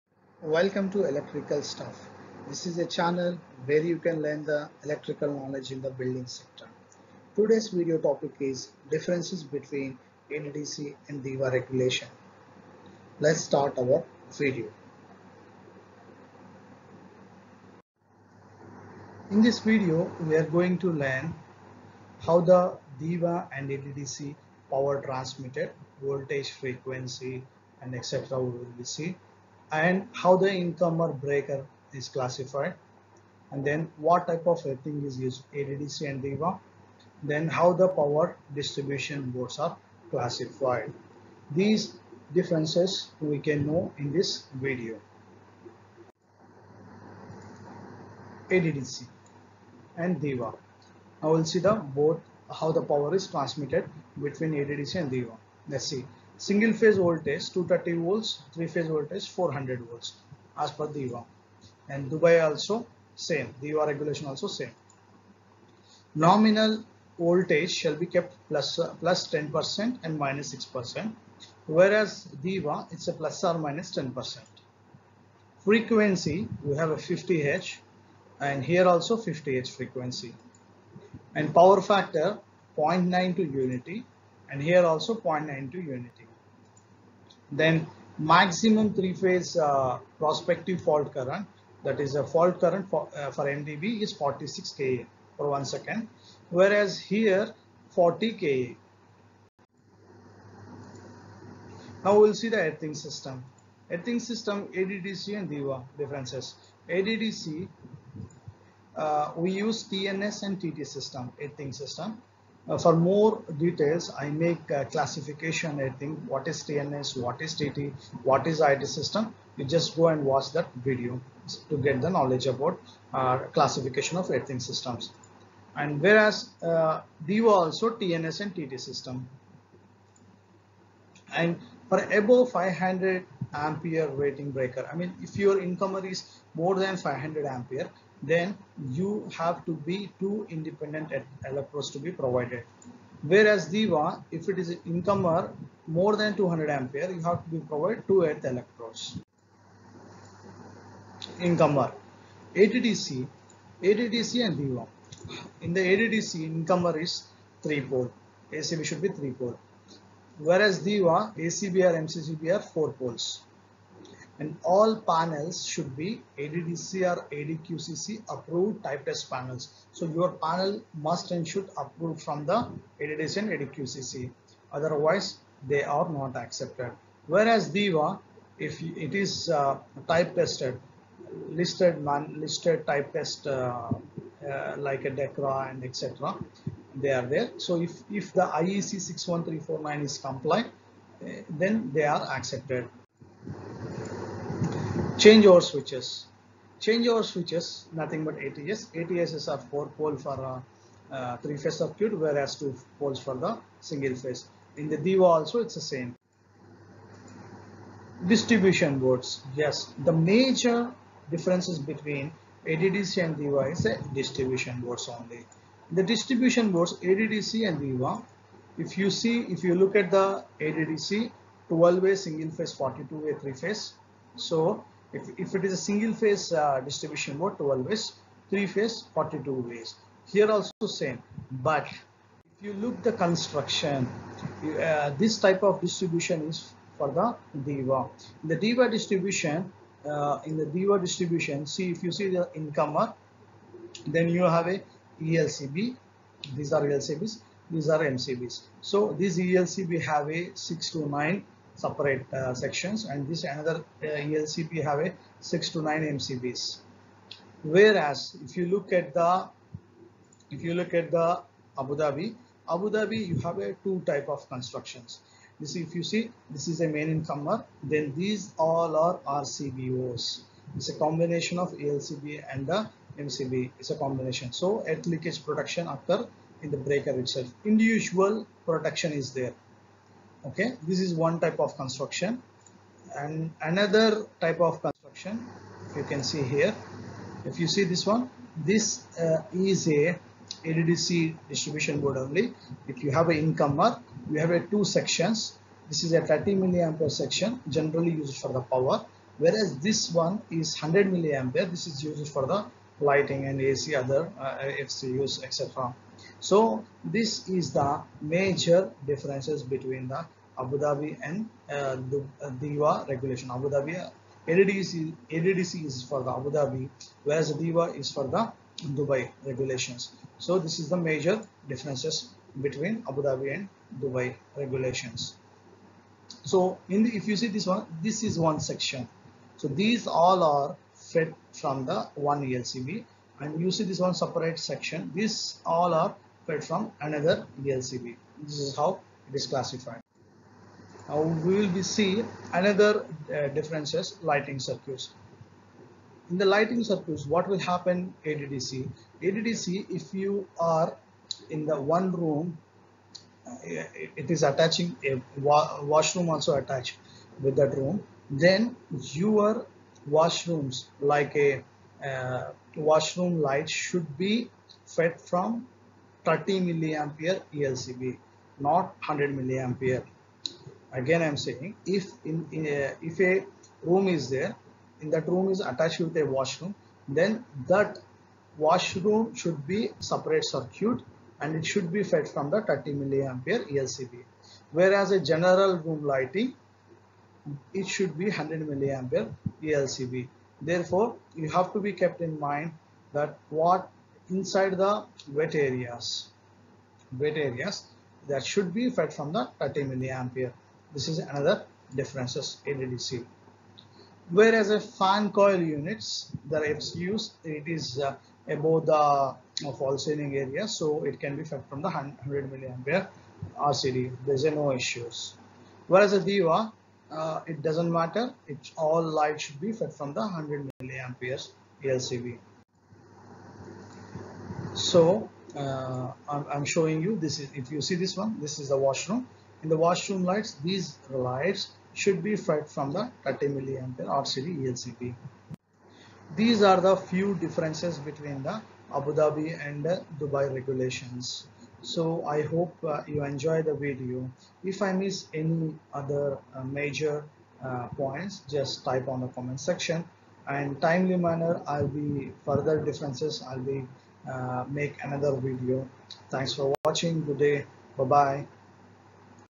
5 Difference between ADDC and DEWA regulations
Dear engineers

Welcome to Electrical stuff ,this is a flat form to share electrical knowledge in building sector

Abu Dhabi distribution company (ADDC ) ,Dubai electricity and water authority are two  major electricity companies in United Arab emirates.

 In this video I like to give  you few difference  between these two distribution companies

1-In this video you will know  voltage, Power factor, frequency etc  of two distribution boards
2-What type of earthing system implemented
3-What type of  switchgear & incomers are preferred
4-How distribution boards classified in ADDC and DEWA

So watch the video till the end to know these differences

Amazon link to purchases :Electronics